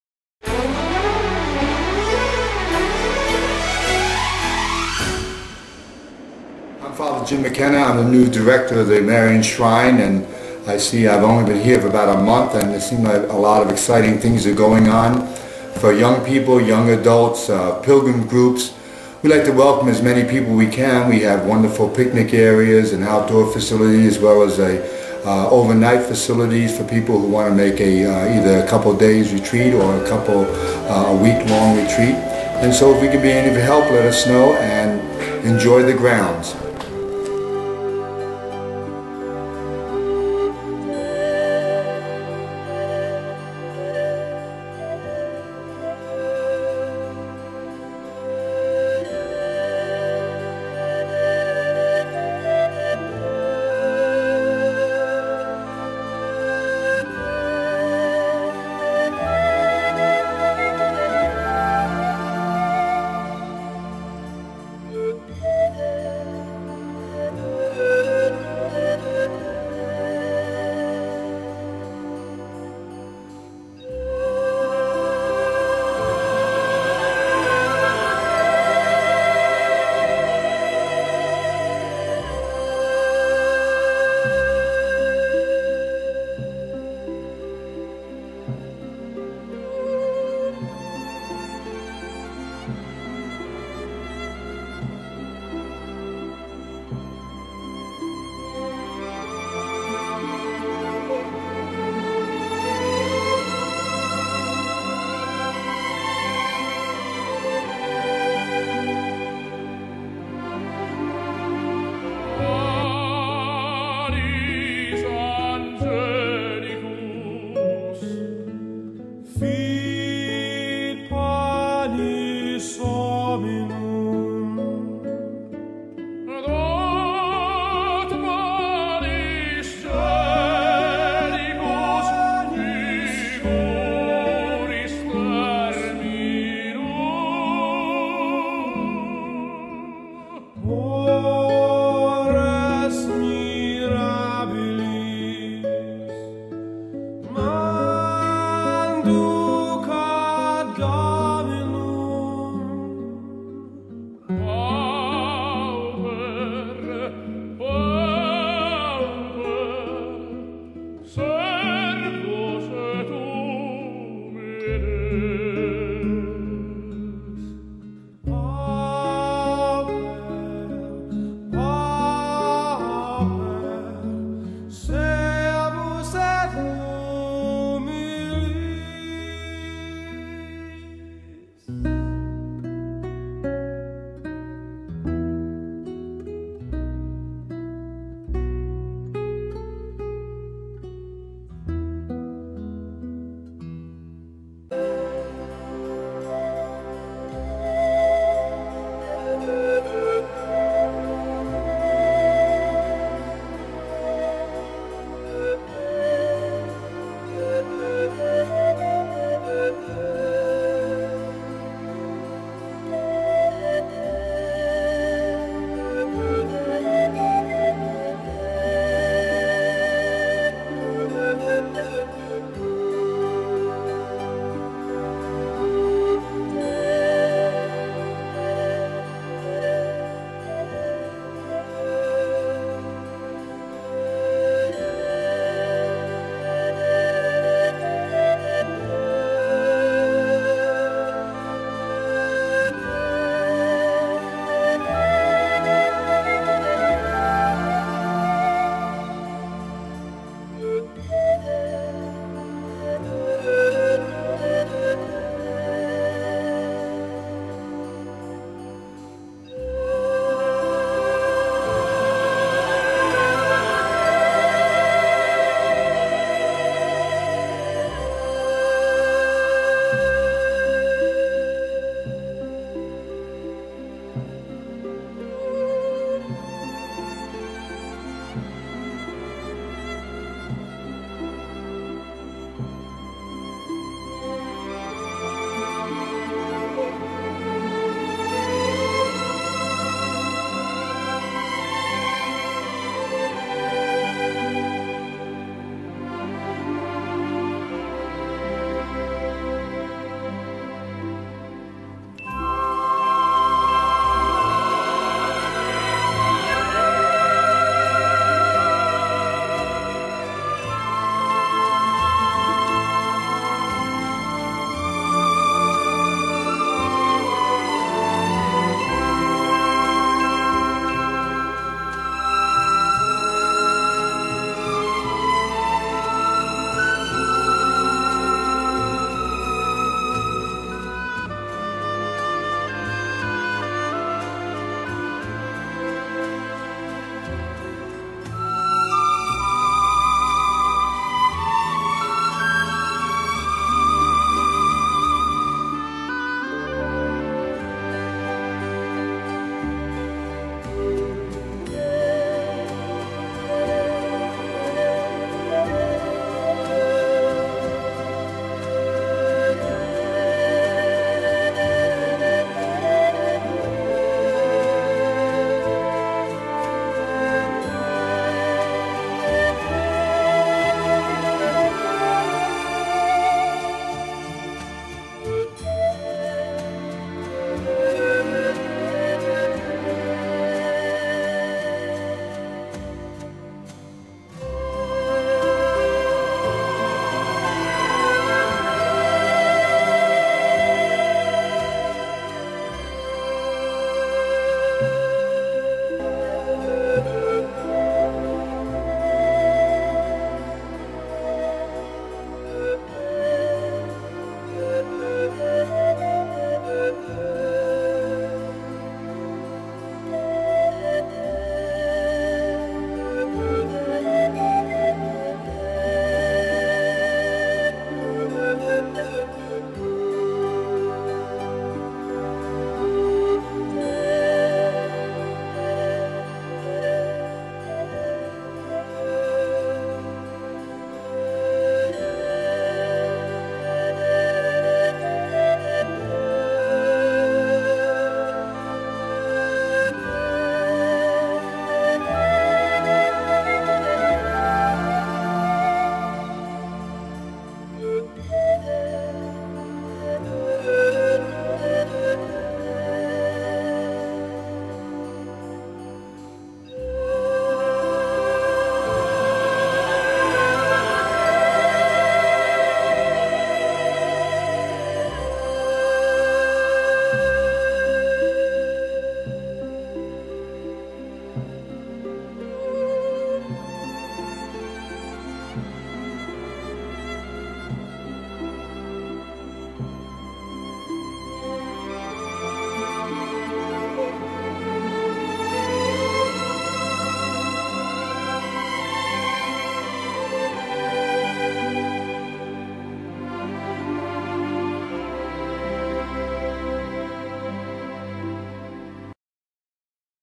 Marian Shrine - Stony Point, NY.wmv
A short video showing the grounds, the chapels and smaller devotional shrines at the Marian Shrine